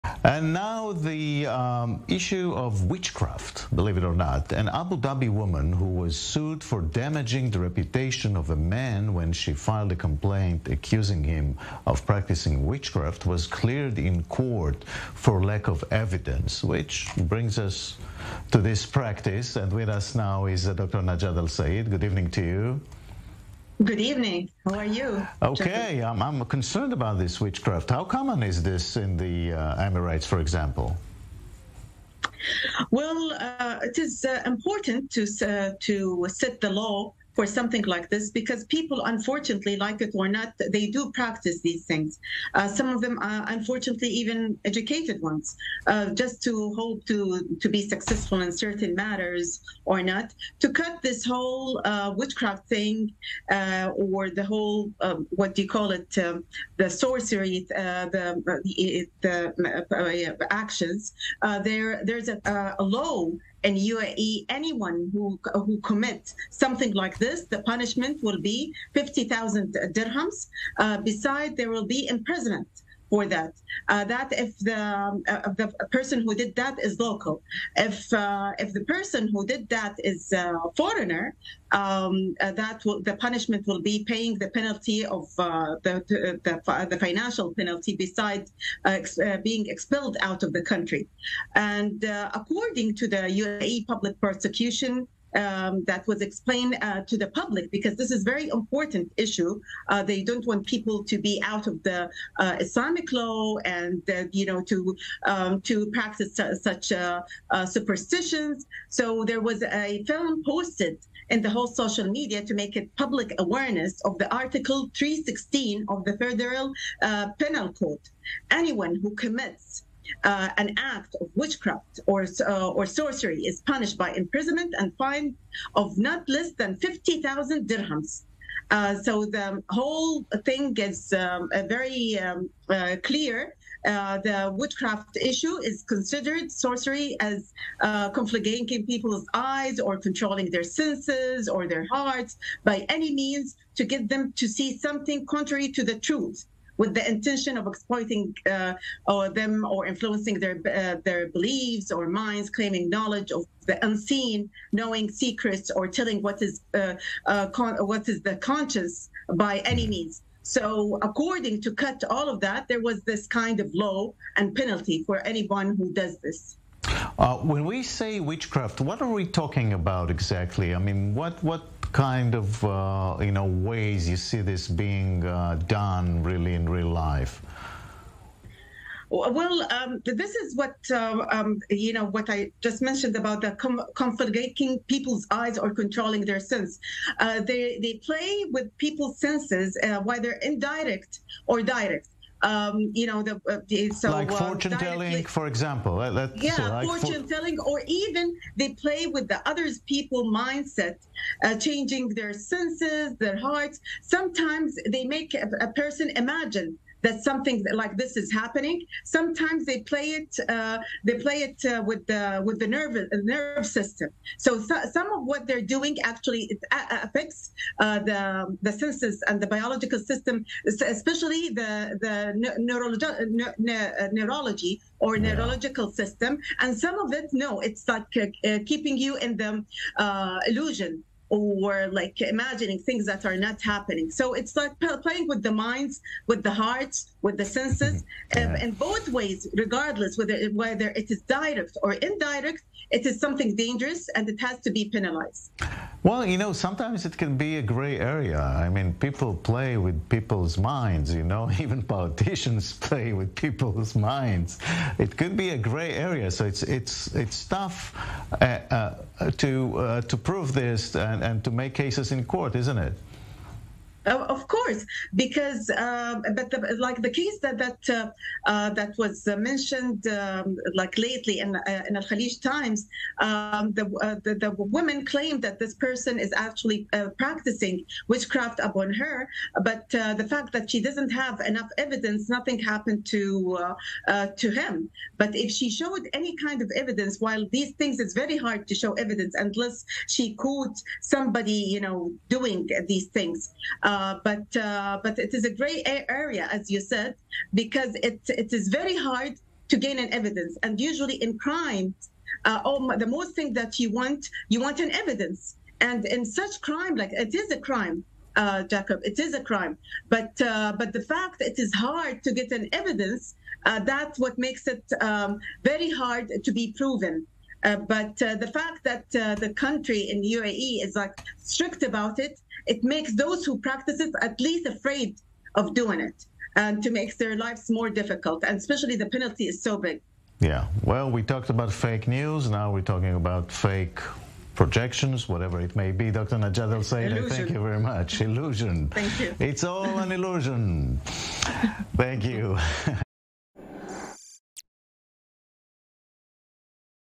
UAE clears woman of moral damages in witchcraft case
Jail time and deportation for witchcraft?

Host Jacob Eilon is joined by Dr. Najat Alsaied to break down why the UAE is taking the act of witchcraft so seriously after a woman was cleared of moral damages in a recent case